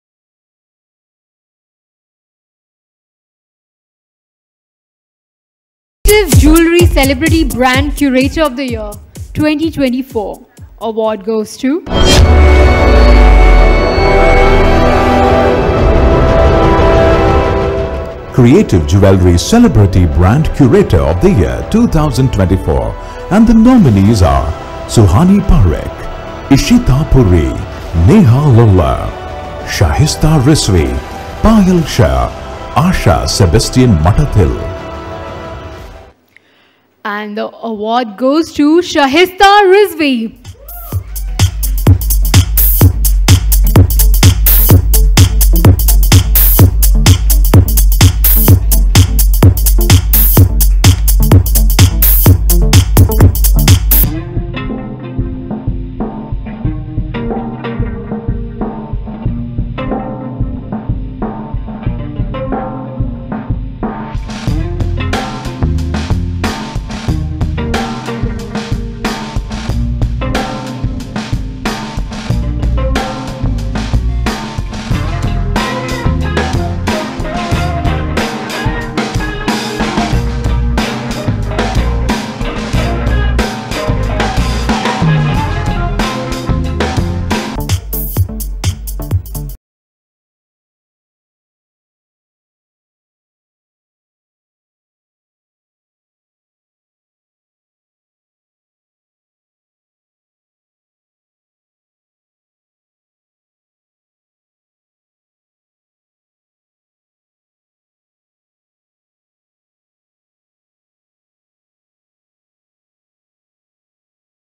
SHAHISTA RIZVI Honoured at the 13th Annual India Leadership Conclave Power Brand Awards 2024
SHAHISTA RIZVI was honoured with the prestigious Creative Jewellery Celebrity Brand Curator of The Year 2024 at the 13th Annual India Leadership Conclave & Indian Affairs Power Brand Awards 2024.